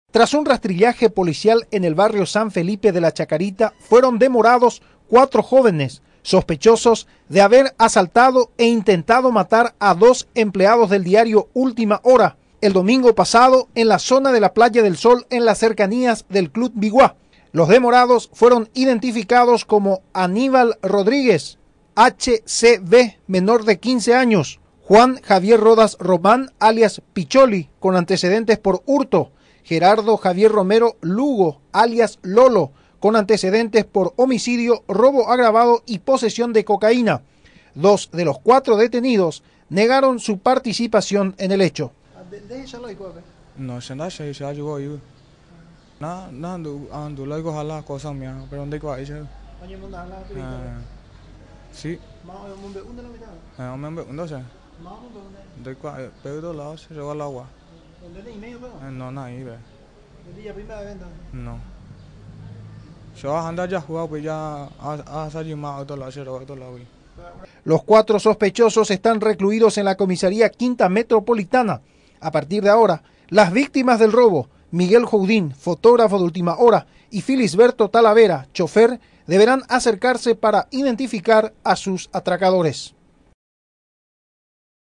Detienen a sospechosos que robaron a empleados de Ultima Hor
Detienen a sospechosos que robaron a empleados de Ultima Hora